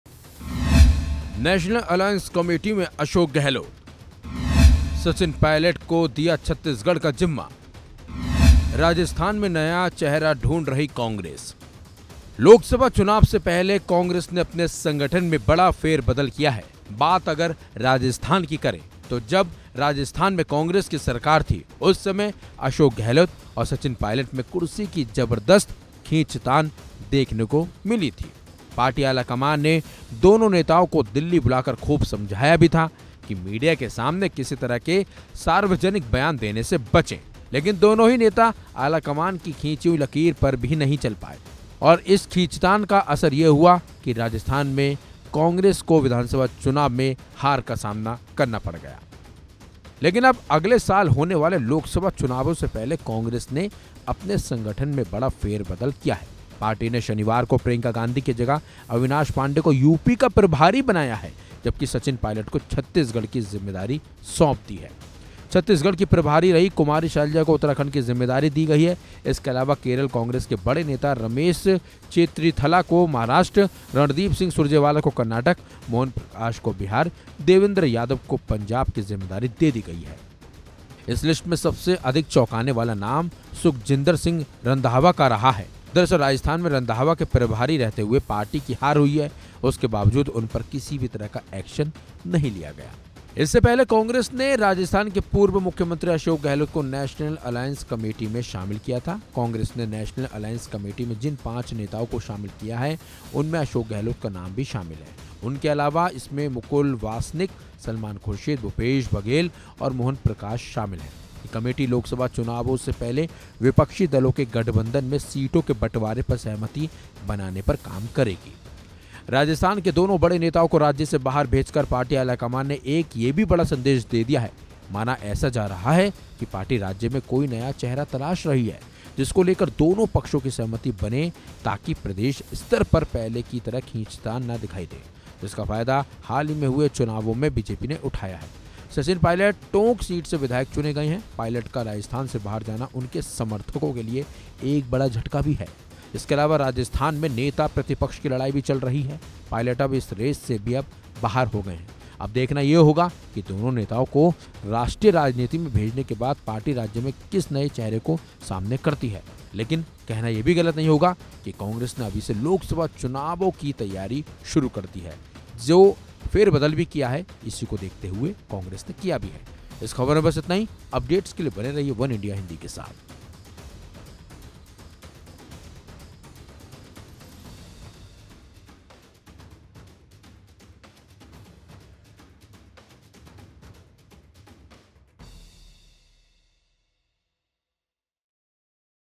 Congress ने हार के बाद Rajasthan से Sachin Pilot और Ashok Gehlot को किया आउट | वनइंडिया हिंदी
कांग्रसे (Congress) ने अपने संगठन में बड़ा फेरबदल किया है. जहां एक तरफ अशोक गहलोत (Ashok Gehlot) को नेशनल अलायंस कमेटी (National Alliance Committee) में शामिल किया गया है. तो वहीं दूसरी तरफ सचिन पायलट (Sachin Pilot) को भी राजस्थान (Rajasthan) से आउट करते हुए छत्तीसगढ़ (Chhattisgarh) का जिम्मा सौंपा गया है